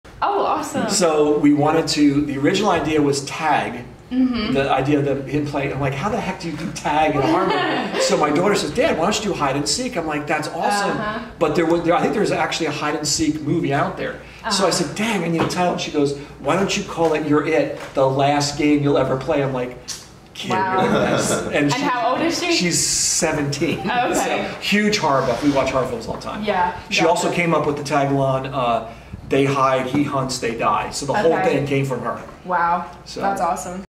Director talks about inspiration behind his new movie filmed in Hickory
Director of You're It, Ron Rossmann, talks about how his daughter helped him name his upcoming movie that will be filmed at Lake Hickory Haunts. Video by Caroline Wills, Record.